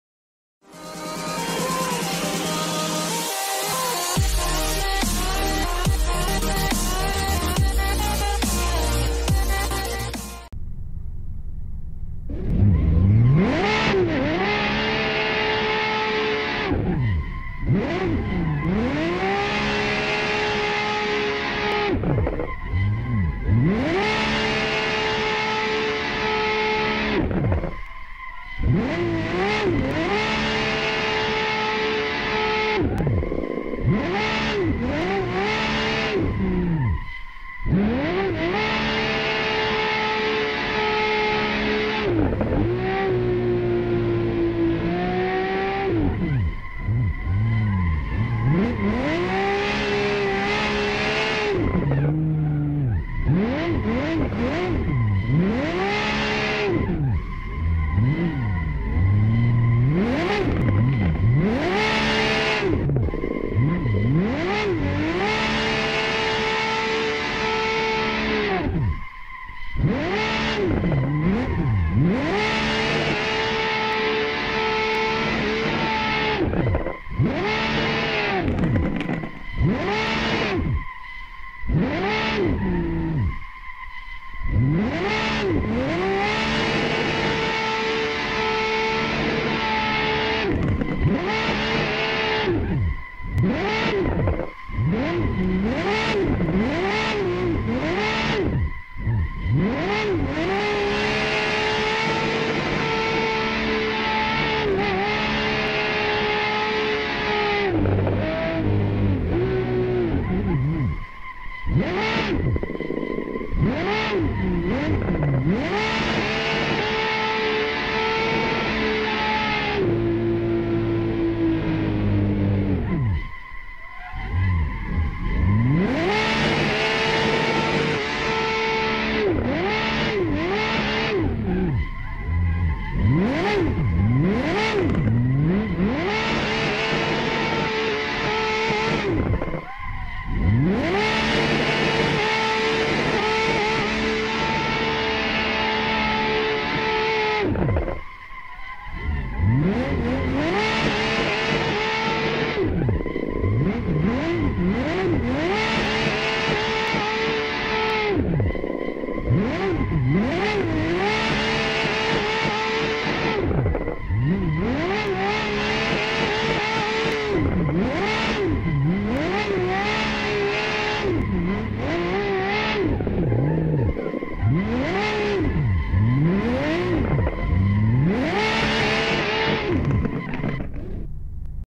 Yokohama Docks Freestyle
Freestyle drifting a lambo around Yokohama docks